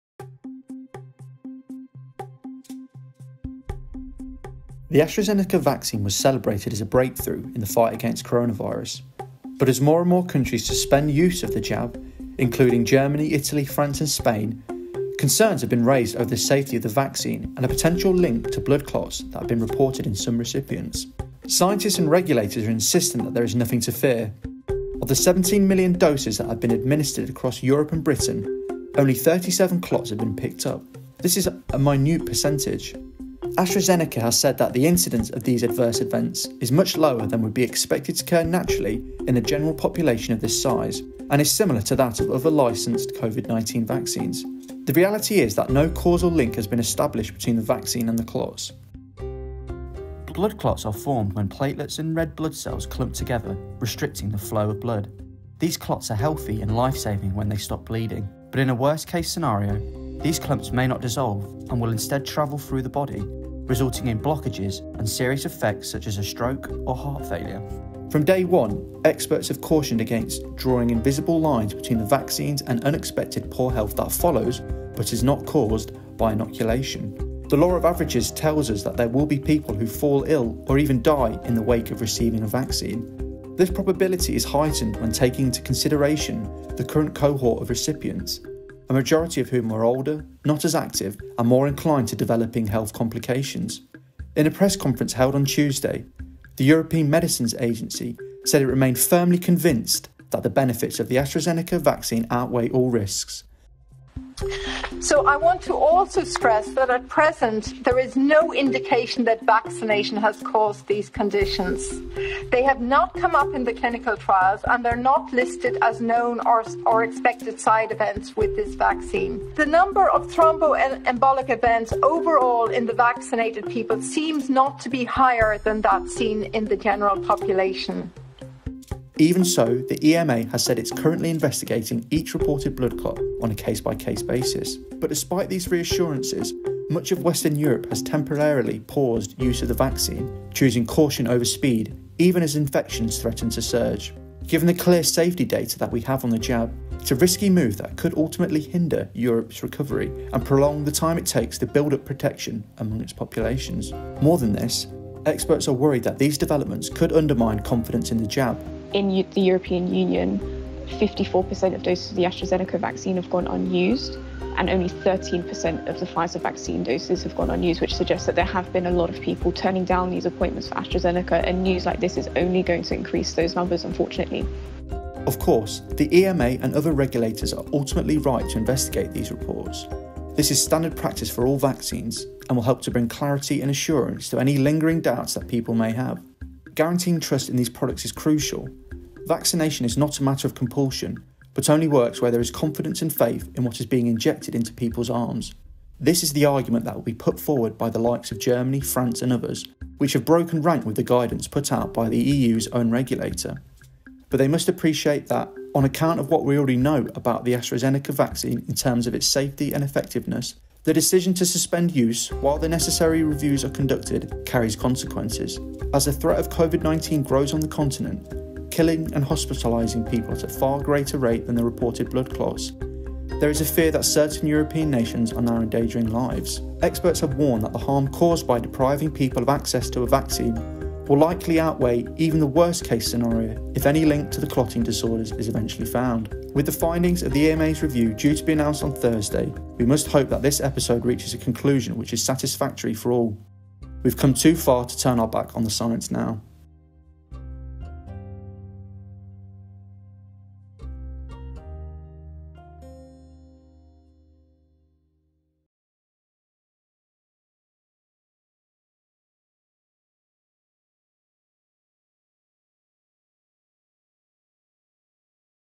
Should you worry about the AstraZeneca vaccine?
As more and more countries in Europe suspend the rollout of the AstraZeneca vaccine, fears have been raised over the jab and a potential link to dangerous blood clots.

As it stands the European Medicines Agency and World Health Organisation have stated that there is no link between the vaccine and blood clots. But how did these fears arise, and what could a delay in the vaccine rollout mean in the fight against Covid-19?

The Independent's science correspondent Samuel Lovett explains exactly what is going on with the AstraZeneca vaccine.